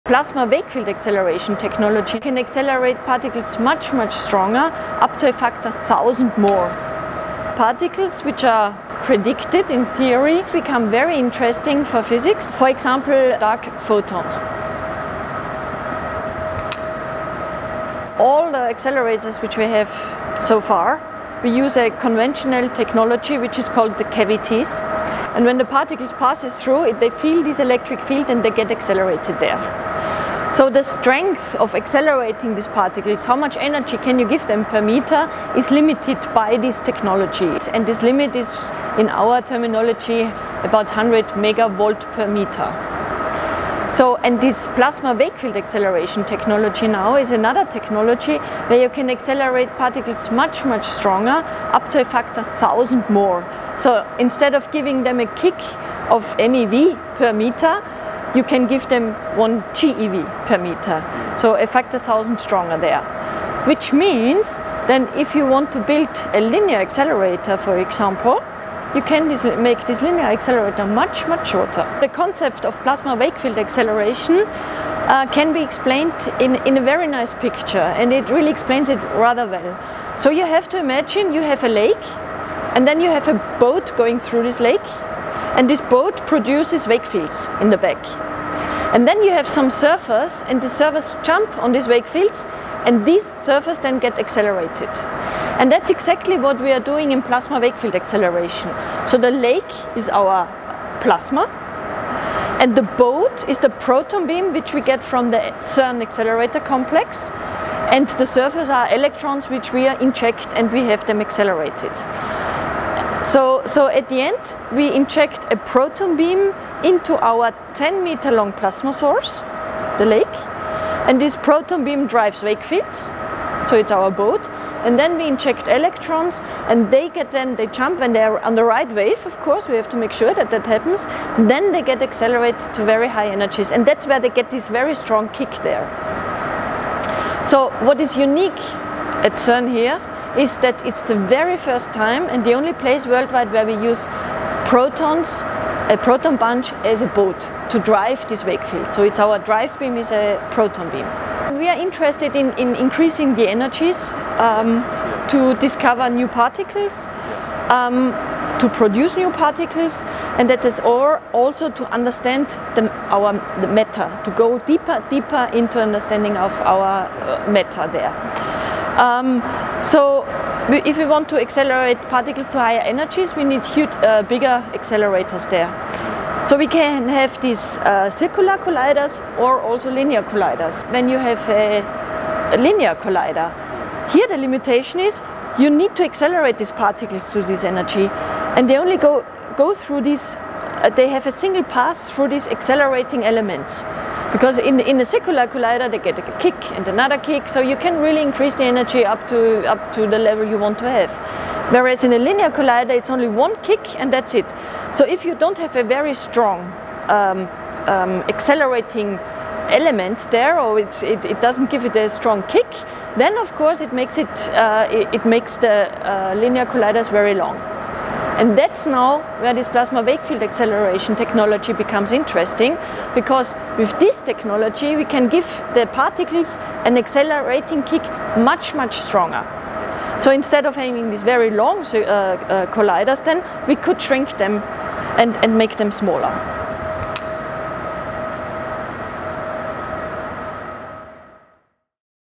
How CERN's wakefield plasma accelerator AWAKE works
A new breed of collider, called plasma wakefield accelerators, can study fundamental physics in new ways by doing something the Large Hadron Collider cannot do: colliding electrons.